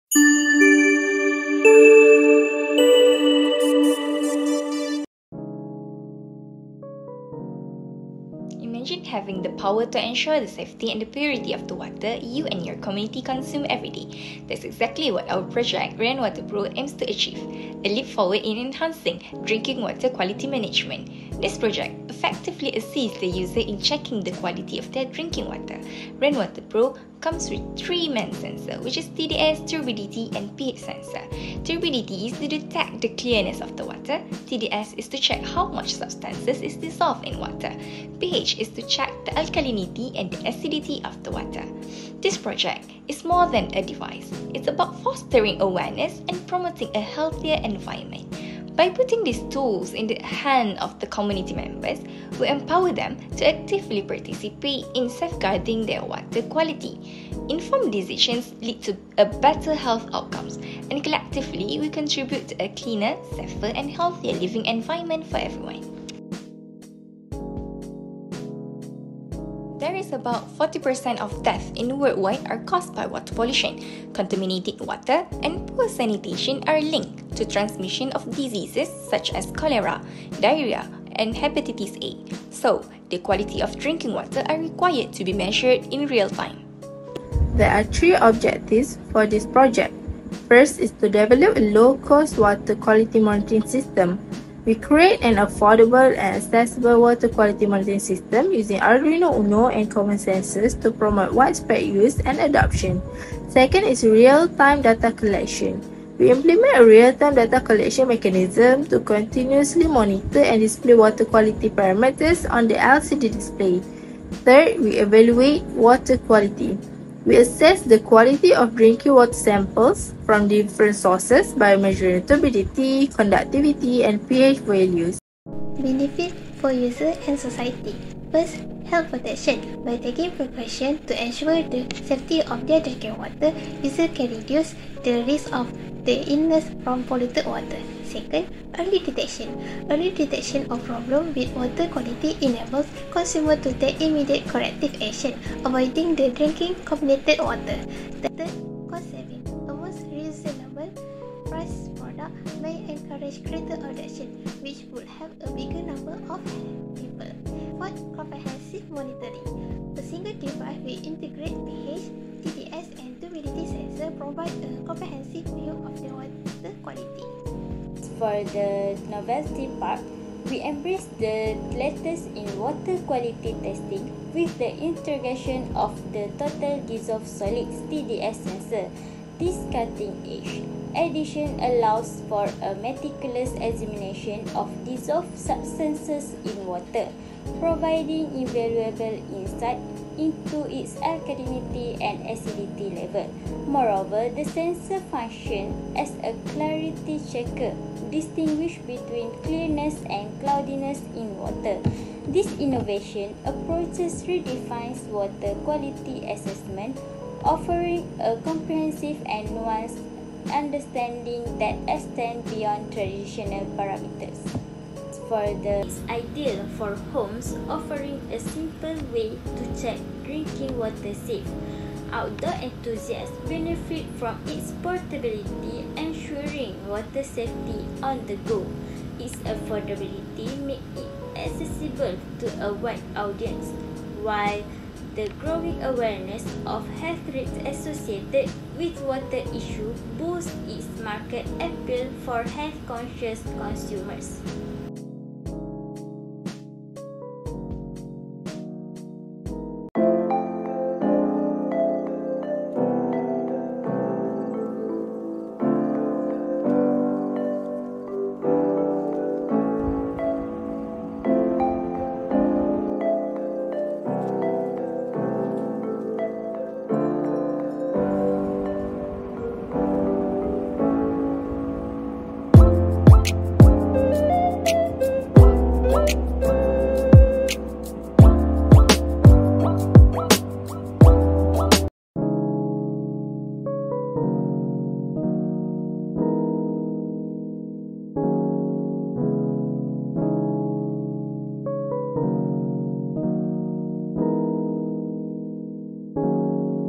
440 RenWater Pro
Participation Category : B: Higher Institution Student
Title : RenWater Pro
Supervisor : Dr. Wan Maisarah Binti Mukhtar
Project Leader : Anis Salsabila Binti Ahmad Hafizullah
Members : Norshamimi Qistina Binti Ahmad Shahir, Siti Aminah Saidah Binti Ahamed Musthafa & Siti Nurlaila Fitrina Binti Che Bisi
Institution : Universiti Sains Islam Malaysia